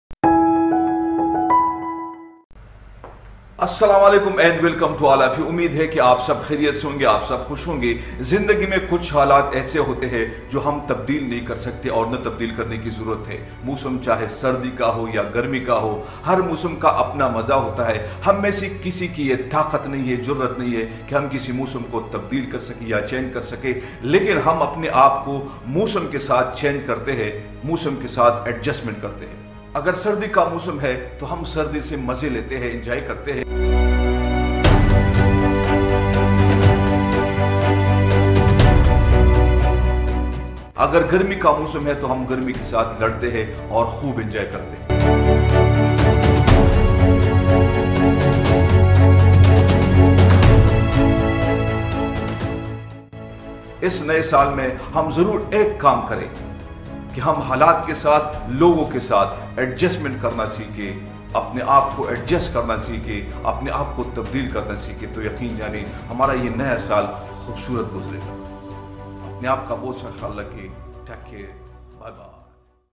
2022 Important message| mental health with Falak Niaz
Important message in 2022, if we follow this message then we can be strong mentally.
Mental health includes our emotional, psychological, and social well-being. It affects how we think, feel, and act. It also helps determine how we handle stress, relate to others, and make choices. Mental health is important at every stage of life, from childhood and adolescence through adulthood.